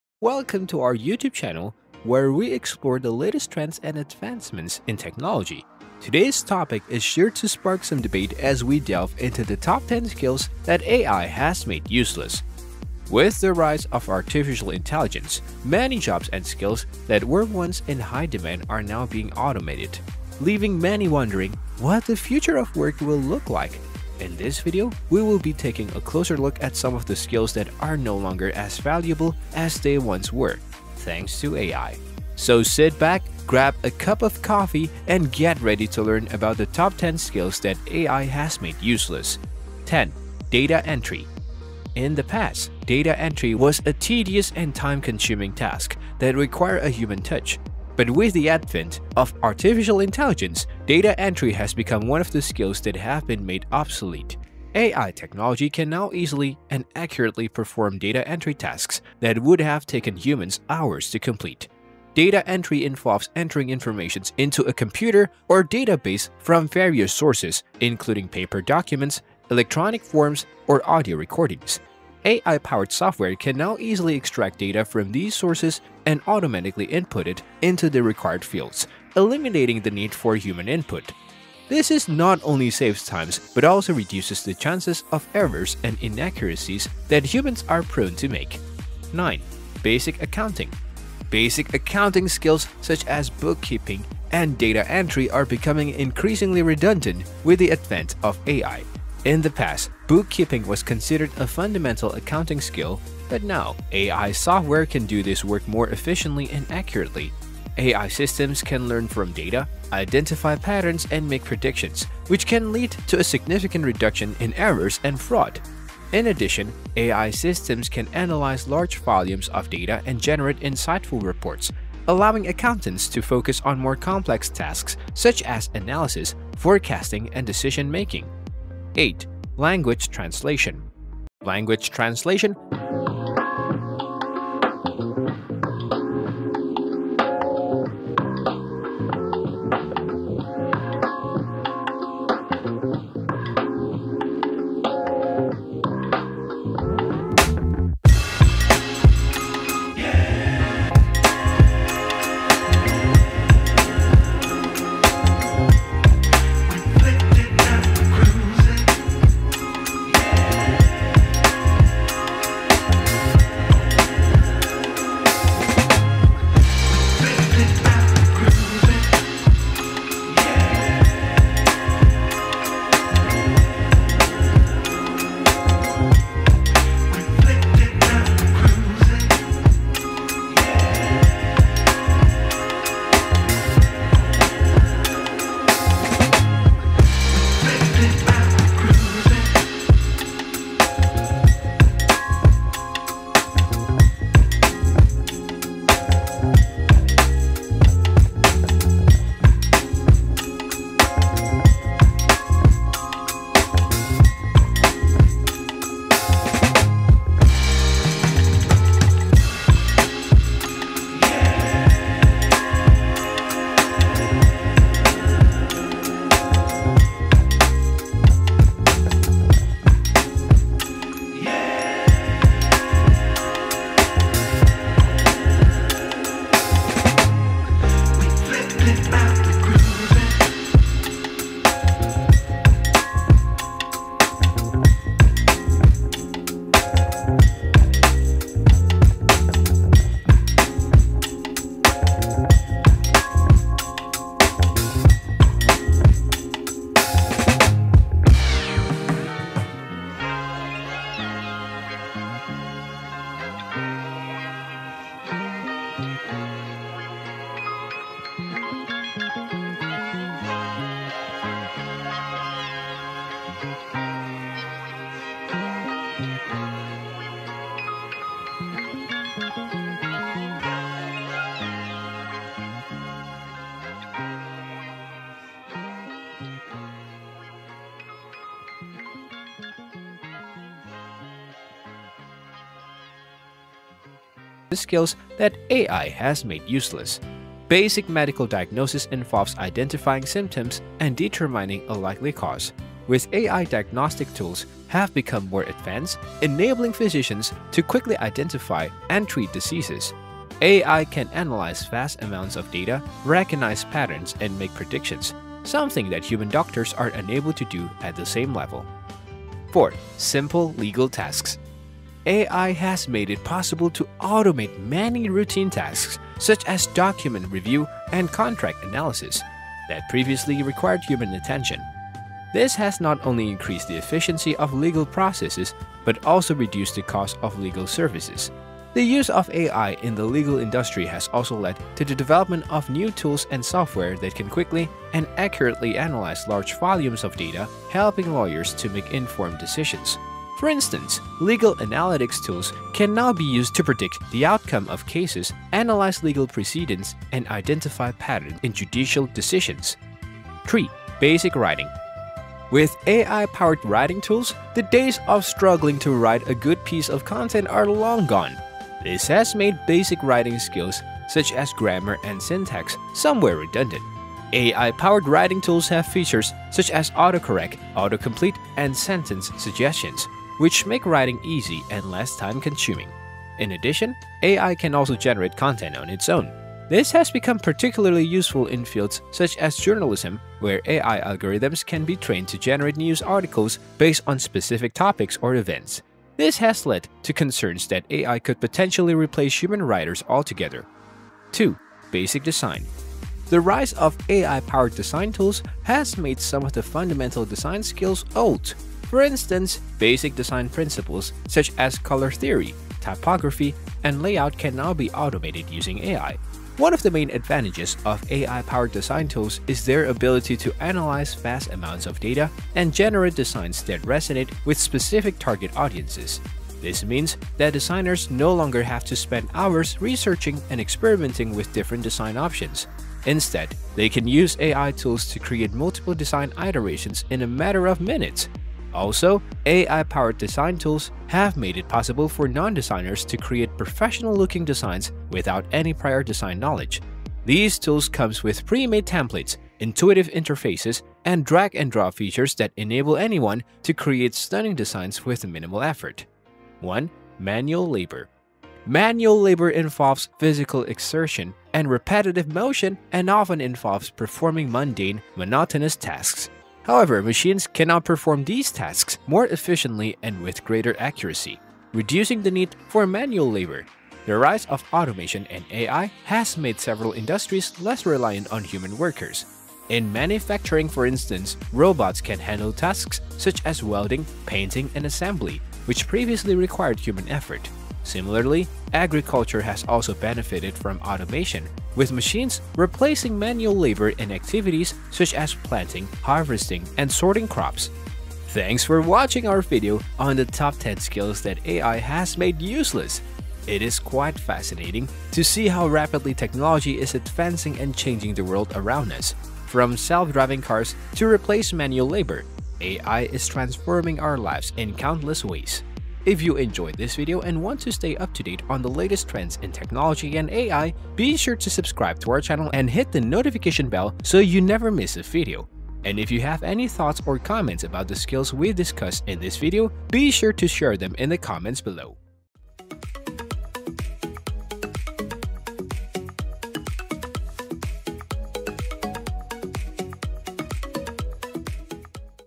The Future of Work: 10 Skills That Will Be Replaced by AI
In this video, we'll discuss the top 10 skills that AI has made useless. With the rise of artificial intelligence and automation, certain skills that were once highly valued have become less important. We'll explore how AI is transforming the job market and what skills are becoming obsolete.